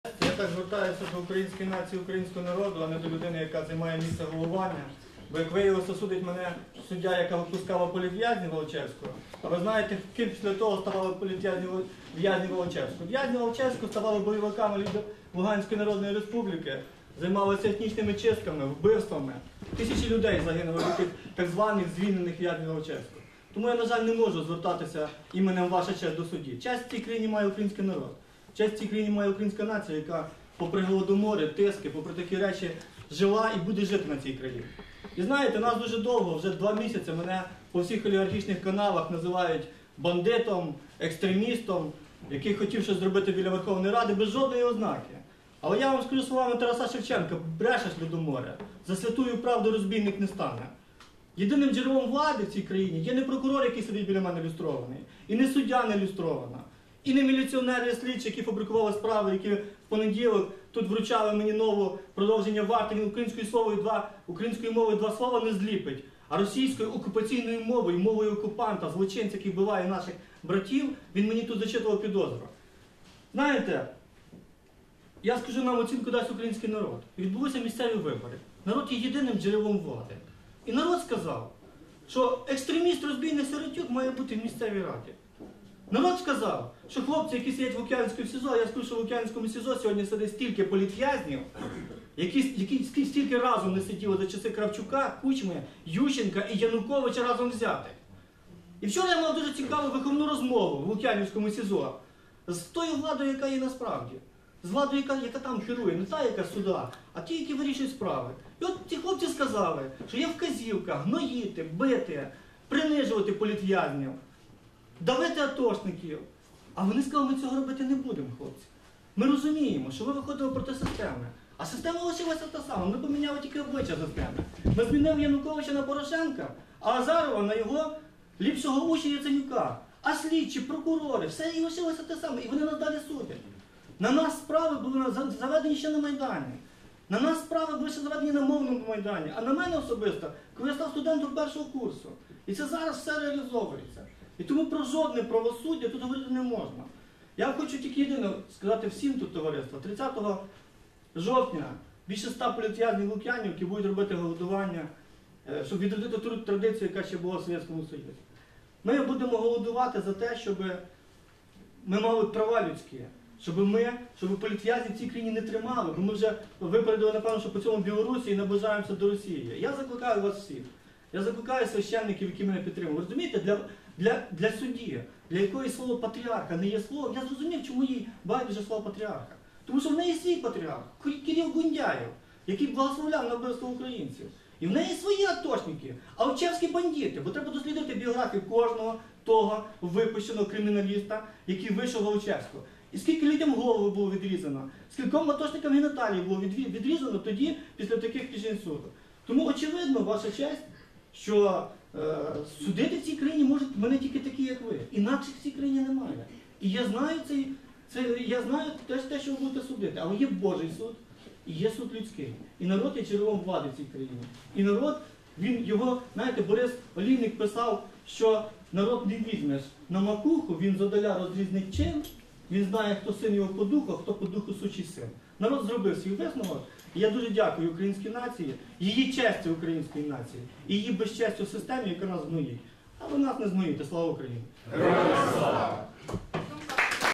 Юрій Сиротюк: Виступ у суді перед тим, як було заявлено про відвід судді Шапутько // 2015
Народ не візьмеш на макуху,
Він зоддаля розрізнить чин:
І хто є син його по духу,
І хто — по духу! — сучий син.